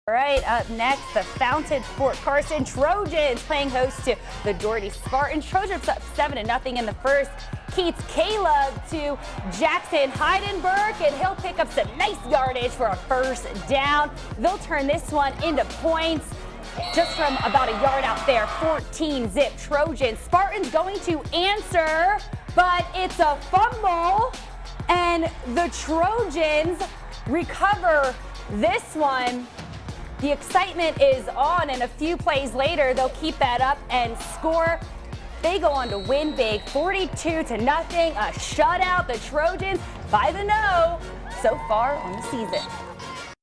Friday Football Fever: Doherty V Fountain-Fort Carson
The Doherty Spartans went on the road to take on the Fountain-Fort Carson Trojans.  The Trojans shut out the Spartans in a 42-0 win.